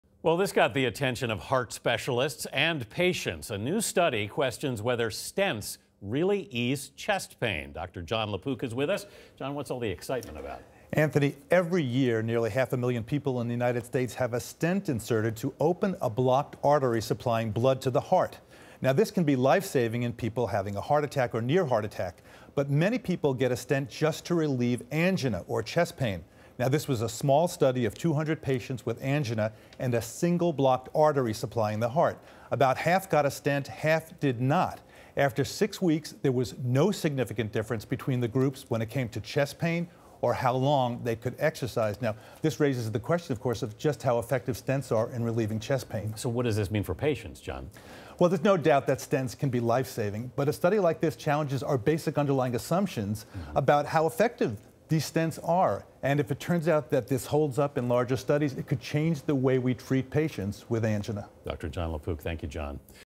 Do stents ease chest pain?
A new study has grabbed the attention of heart specialists and patients. It questions whether stents really ease chest pain. Dr. Jon LaPook discusses the study.